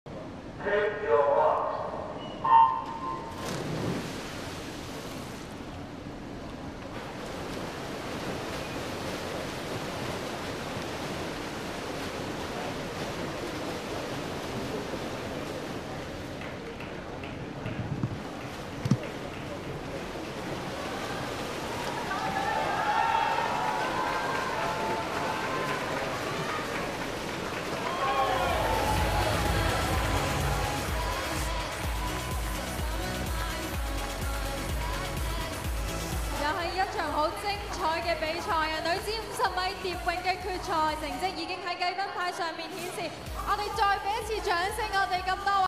JUST 0.24 SECONDS AWAY FROM WR - Swimming World Cup 2017 (race video)
Sarah Sjöström wins women's 50m butterfly.
1. Sarah Sjöström 24.62
2. Ranomi Kromowidjojo 25.46
3. Maaike De Waard 25.79